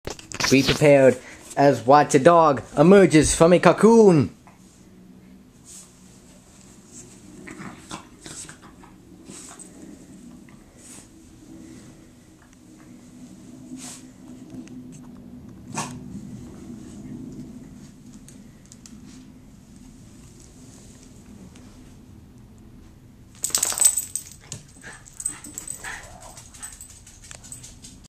My Dog Emerges From A Cocoon
I covered my dog in a blanket and filmed him wriggling and coming out!
SO CUTE!!!